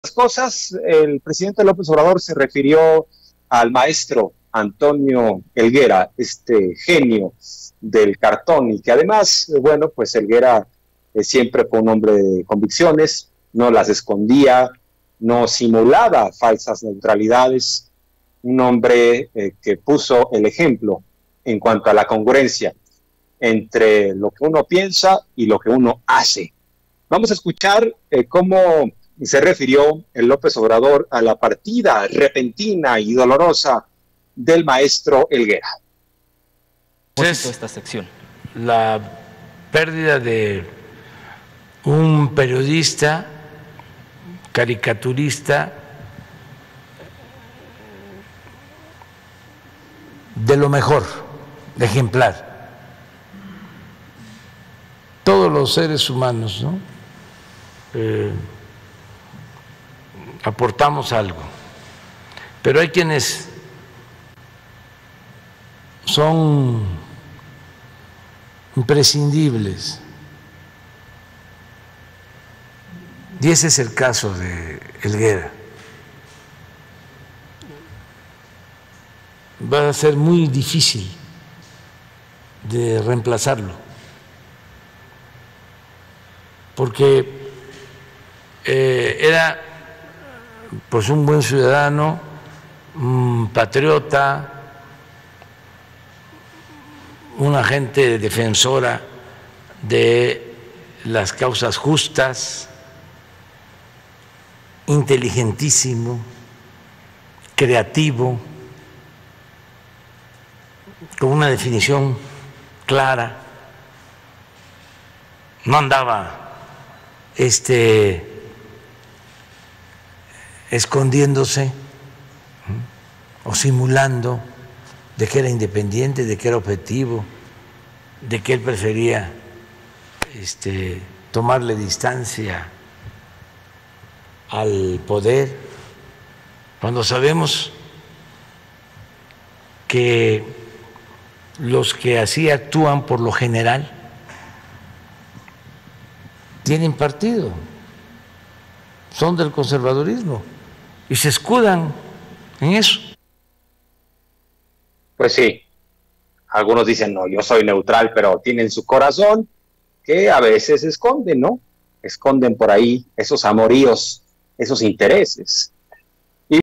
RECONOCE AMLO LEGADO DE HELGUERA
El presidente dice que no se andaba con medias tintas